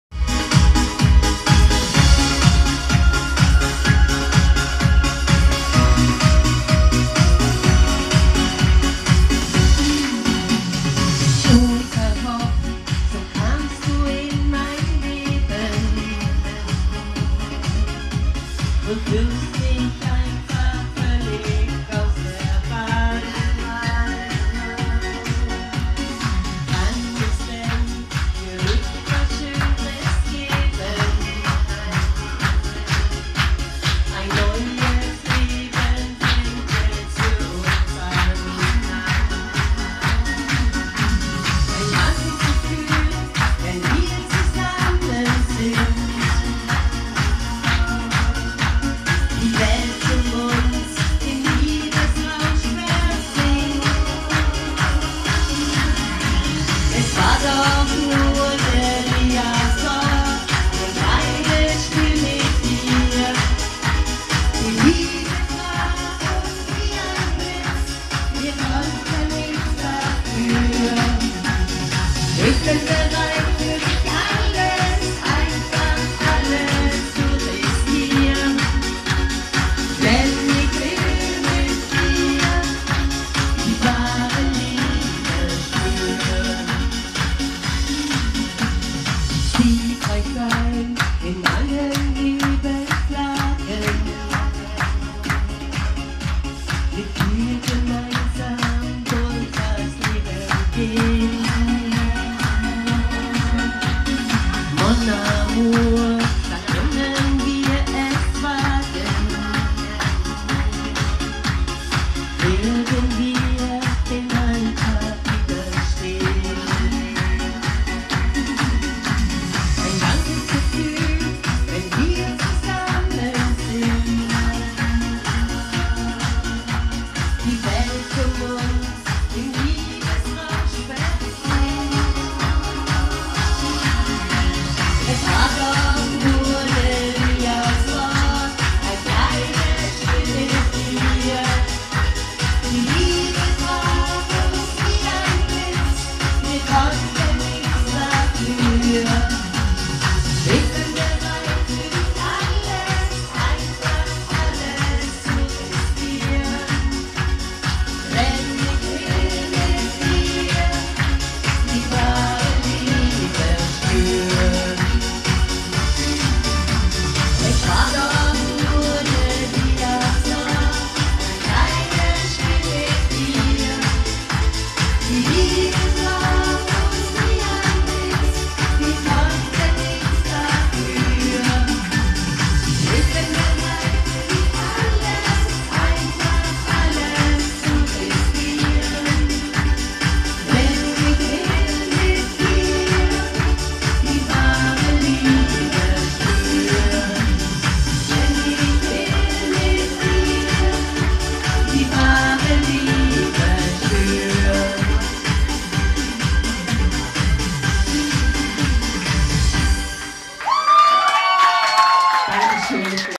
10. September 2017
Lorena Lenz  Life bei ihrer Geburtstagsfeier  in Esslingen